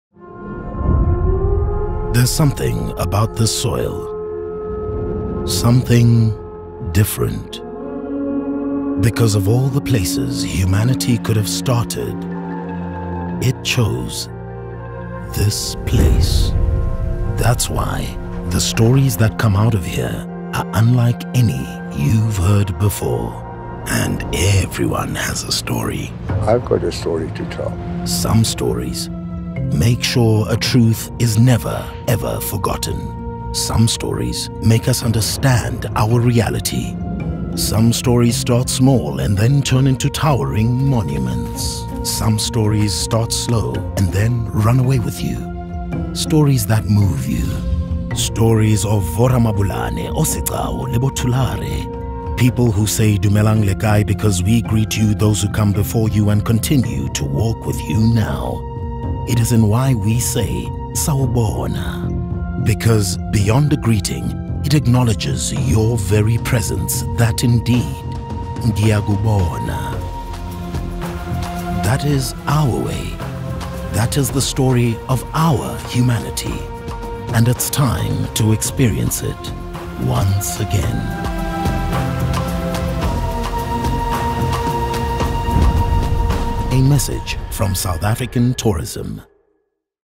South African Tourism: The Greatest Story Of Them All
South African tourism asked us to create an ad showcasing our beautiful country and reminding people that at the end of lockdown, we want them to carry on creating their beautiful stories.